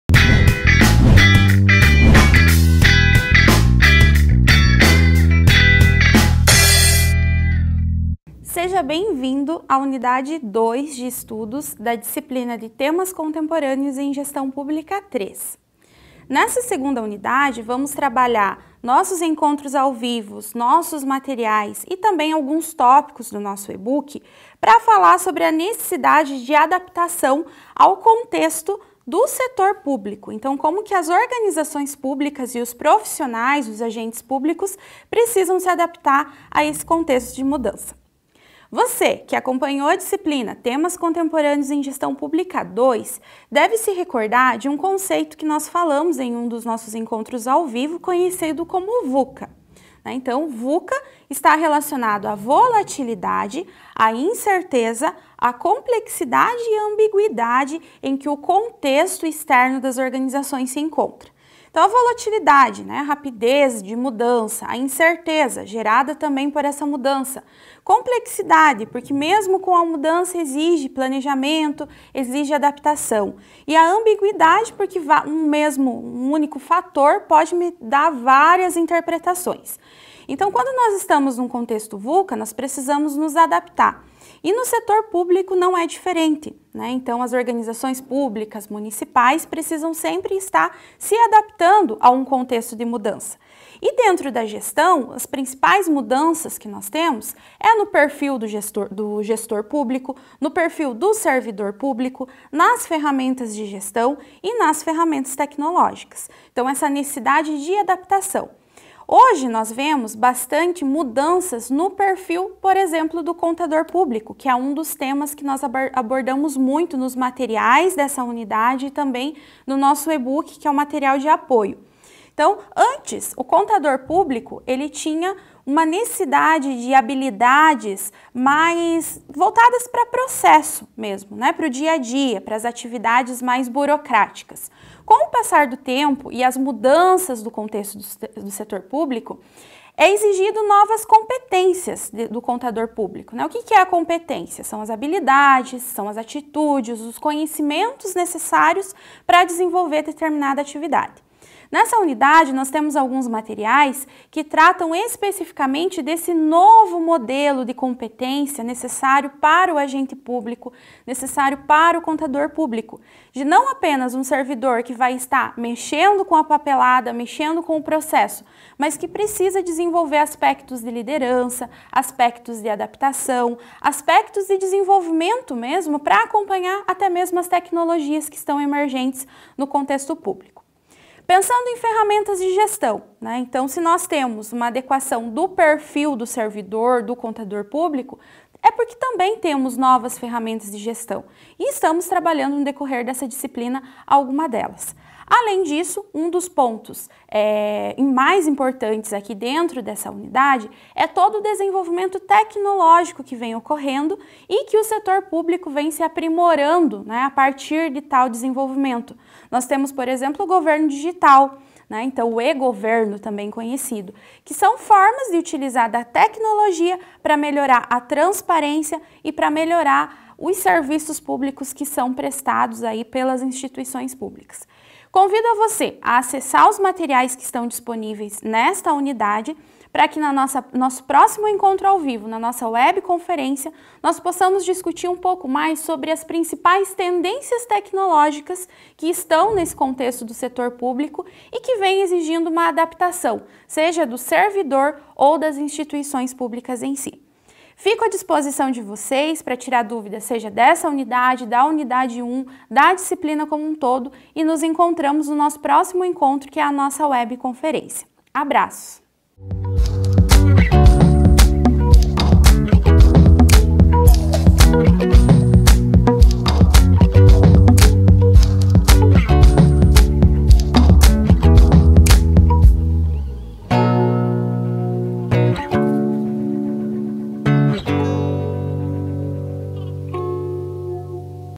[ADMP] - Adaptação ao Contexto do Setor Público
Adaptação ao Contexto do Setor Público de Temas Contemporâneos na Gestão Pública III com a professora Rosmeiri Aparecida Ribeiro Ferras para o curso de Administração Pública - EaD da Universidade Estadual do Centro Oeste do Paraná.